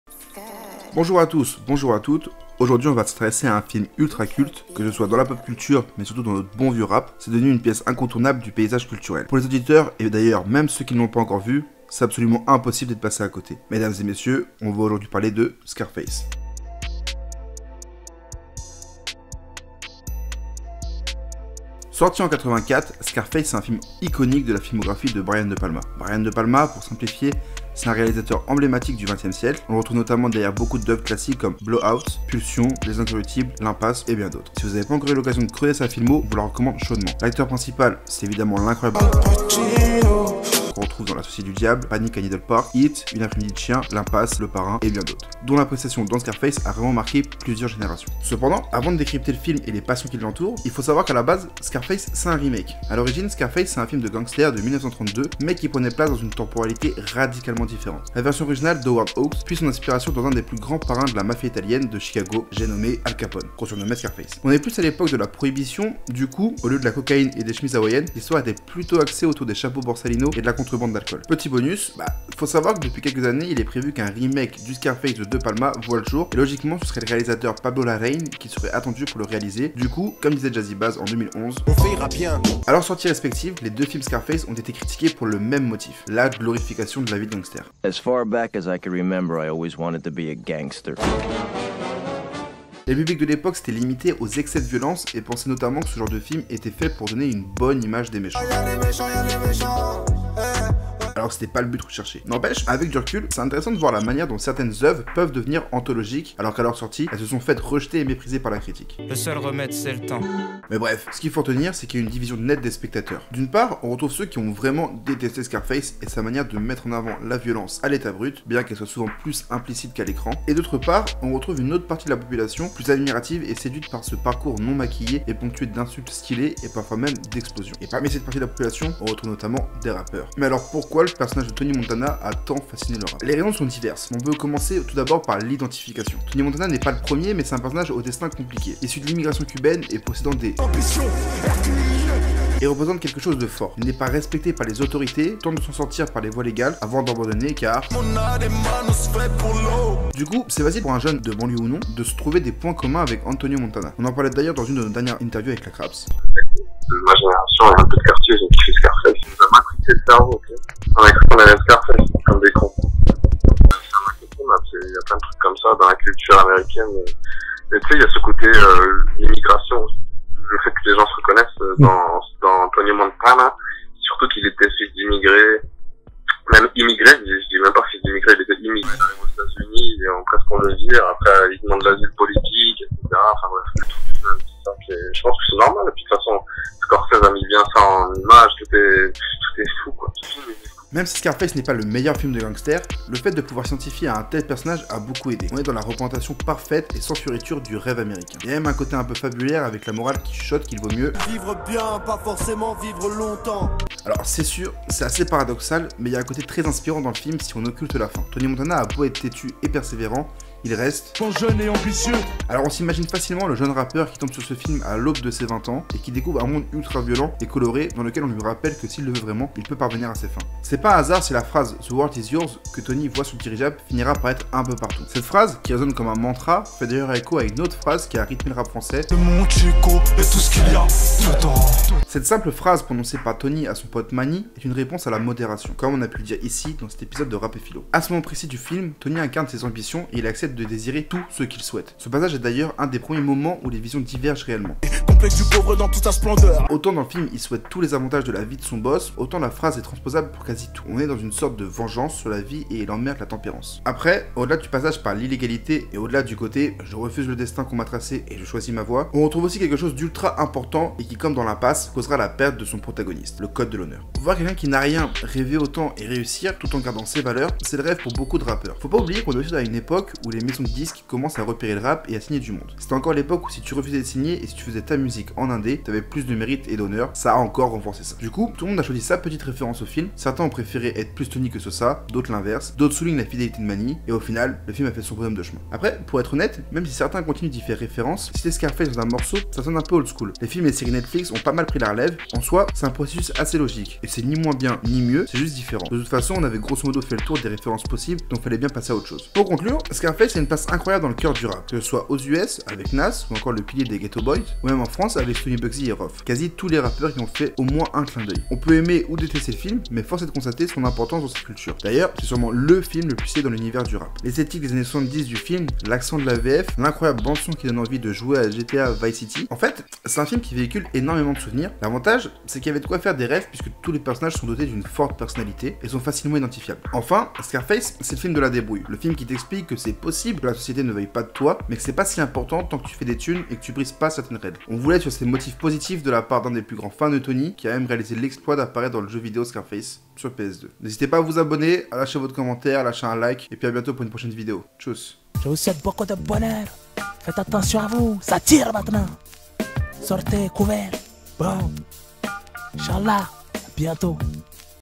RAP & SCARFACE (Reflet d'un idéal) | CUL7URE
Dans cette vidéo, on revient sur un des films les plus cités dans le rap français (et US). Entre références, samples & imitations, ce classique de Brian de De Palma a littéralement boulversé le hip-hop. Du coup, on était obligés d'y consacrer une vidéo.